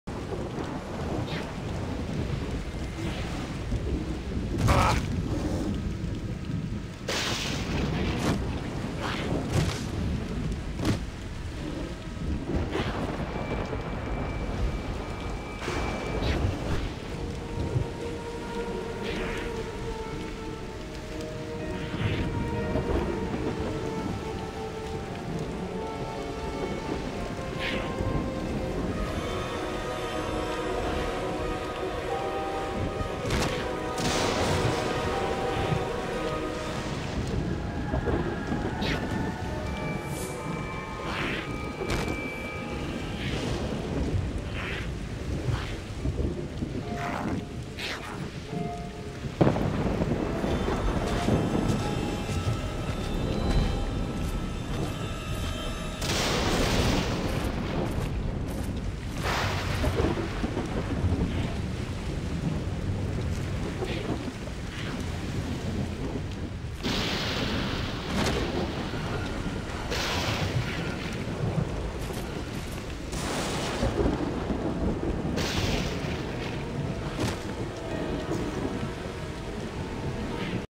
Solution to obtaining the Steppes of Infinity twisted artifact!
It took careful observation and precise jumping to discover the way onto that tallest platform in Steppes of Infinity!